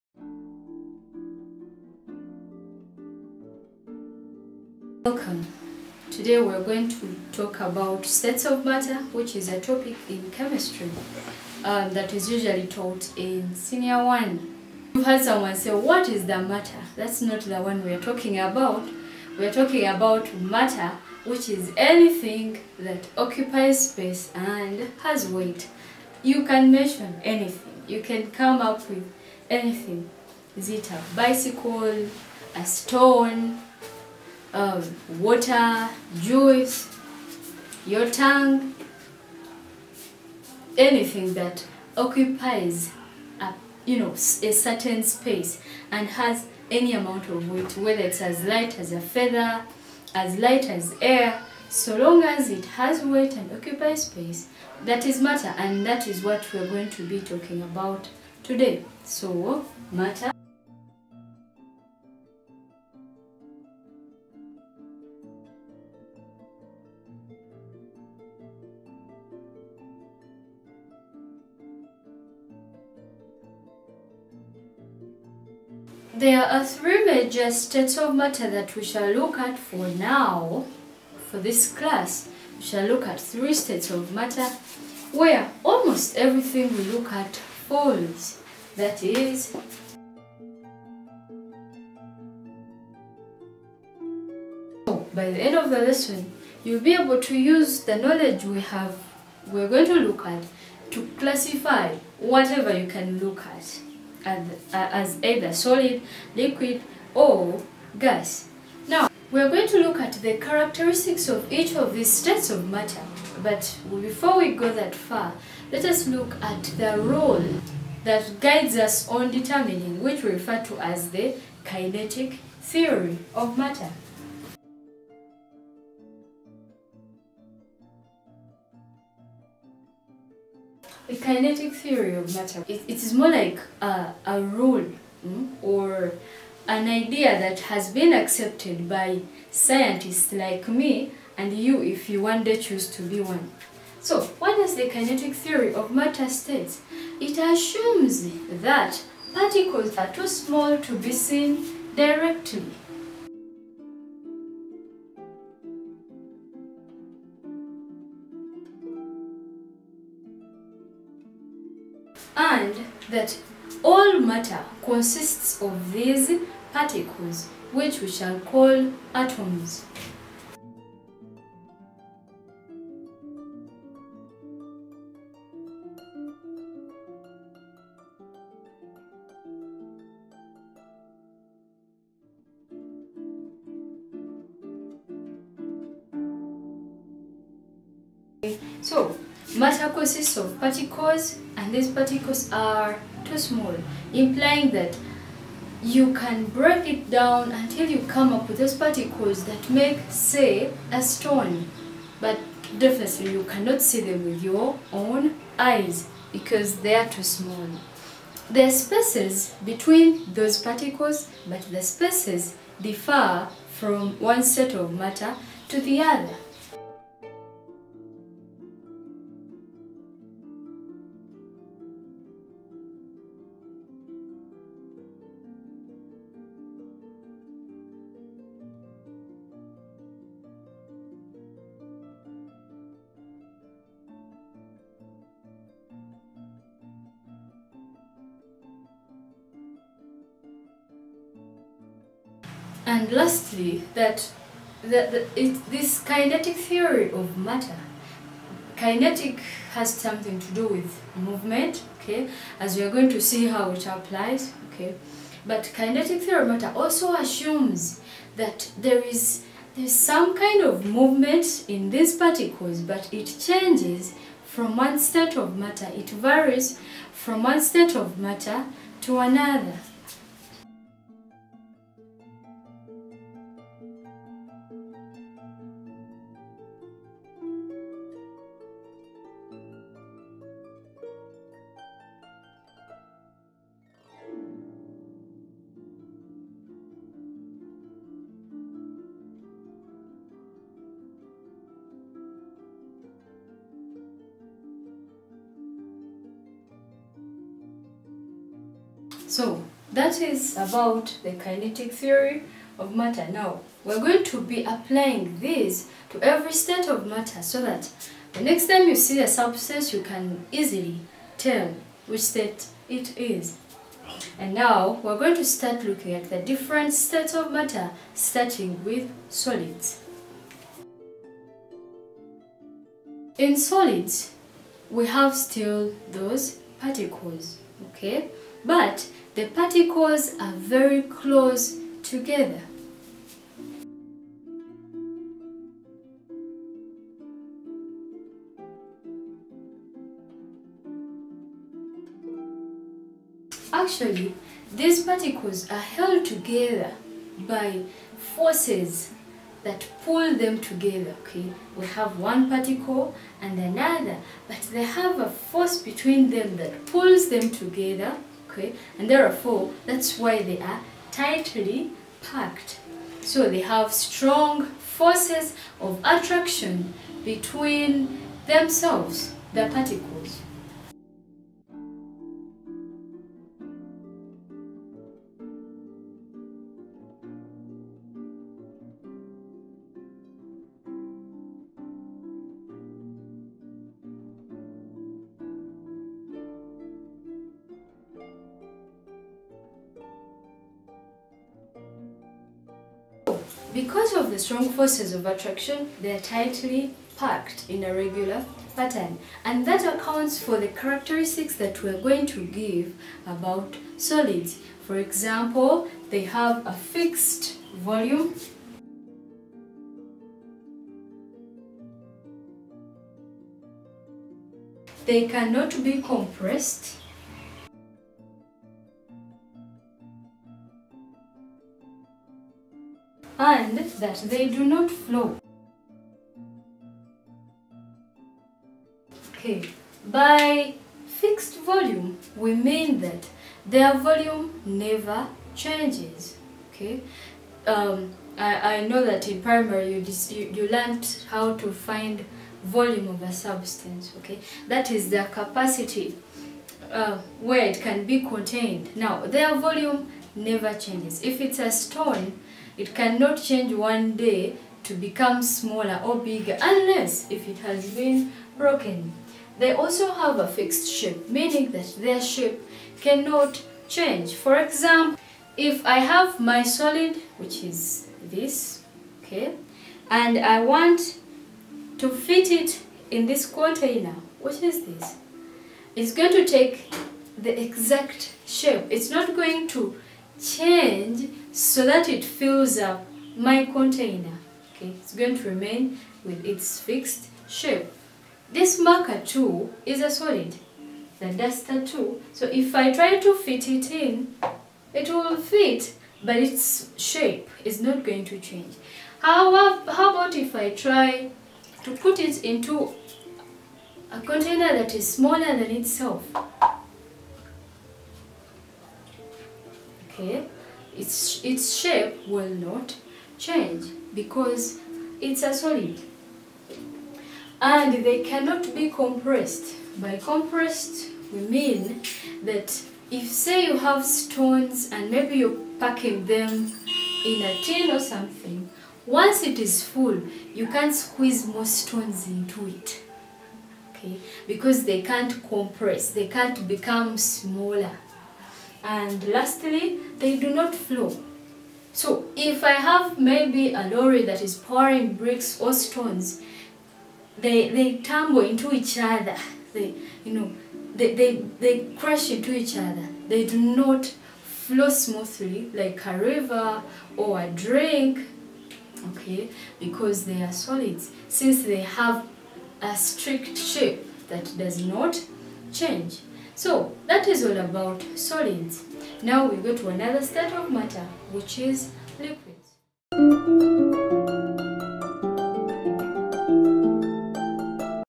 Senior One Chemistry State Of Mater (Solids)
Learn to:
-Define matter
-Mention the states of matter